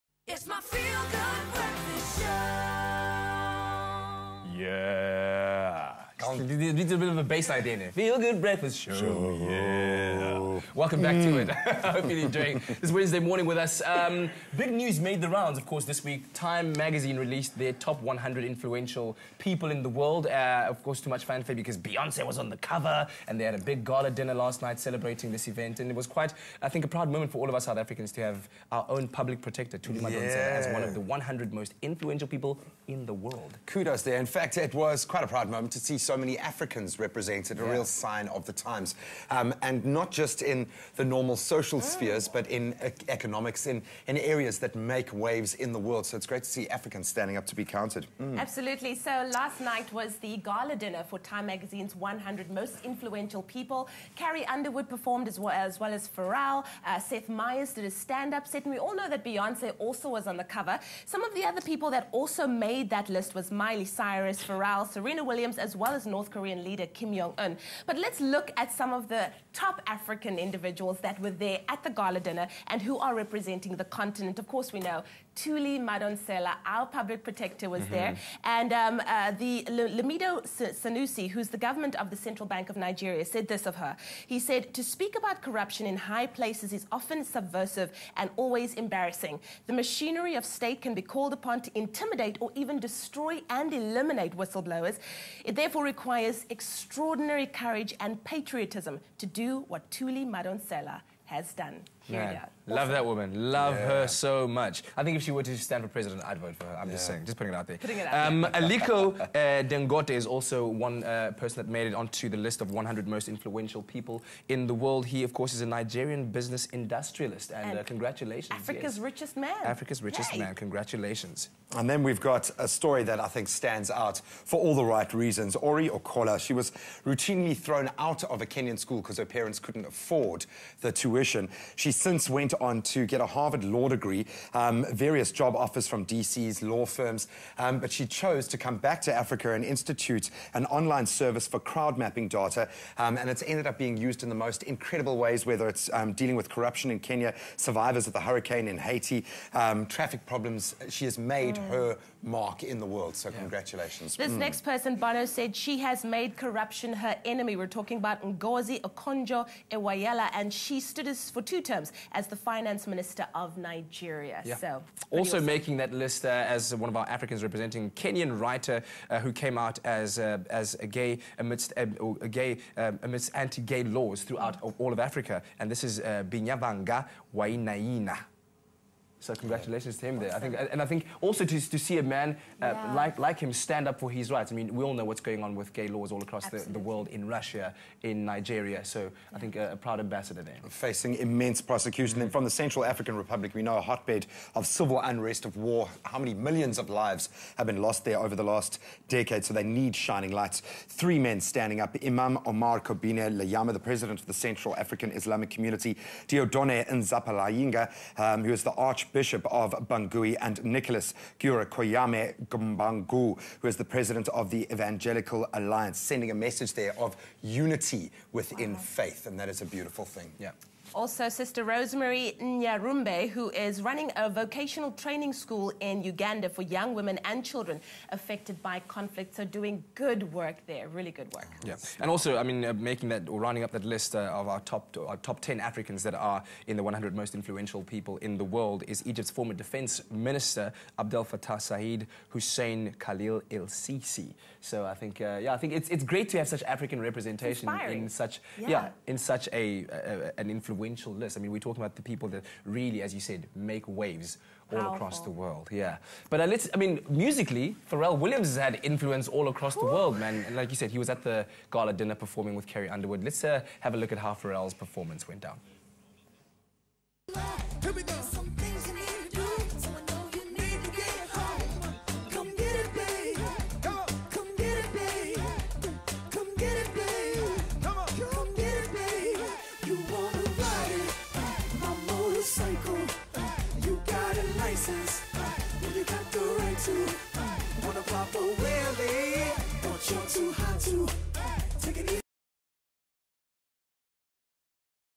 Time's 100 Most Influential Gala (30 April 2014)
2014 TIME 100 gala honours world's most influential people... The list included 10 Africans, including South Africa's fearless public advocate Thuli Madonsela.

We take a look at the top 10 African individuals who made the list. Well done Thuli!